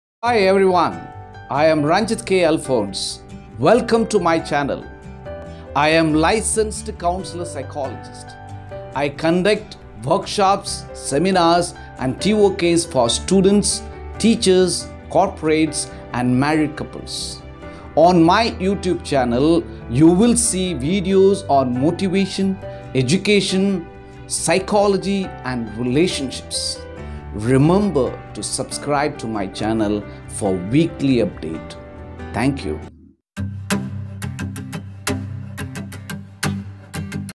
Psychology relationship motivational videos/ YouTube Channel Trailer Ranjith Alphonse Psychologist
Hi everyone
I am Ranjith K.Alphonse certified counselor psychologist.
A trainer, persuader, motivator, mentor, therapist, educator and family counselor with a decade of extensive  experience .
Ministering as:
1) School counselor and mentor
2)Children and youth counselling
3) Marriage and family counselling
4) Lecturer on Psychology ,counselling and motivation